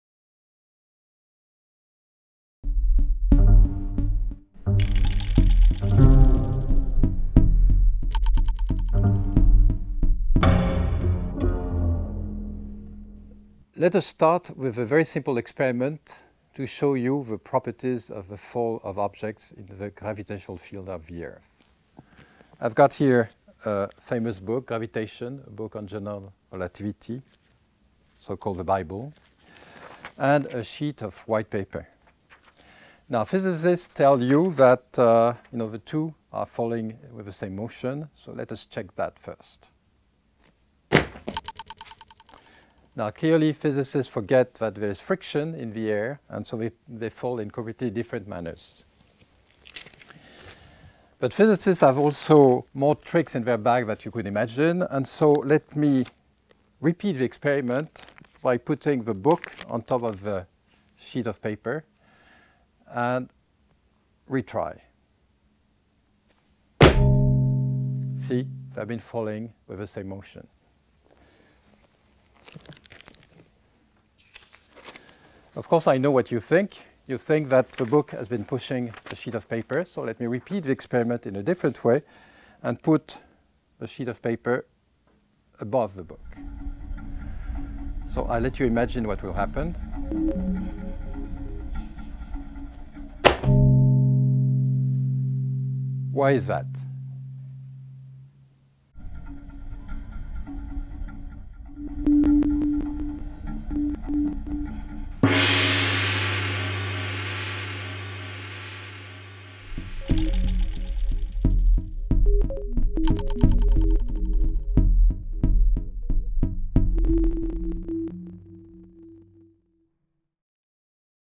Gravity! The book
Book experiment (week I) of the course Gravity!
A course by Nobel Prize George Smoot and Pierre Binétruy
2015
Director: Jean-Luc Robert
Music and sound design: Gorka Alda
Coordination: Marie Verleure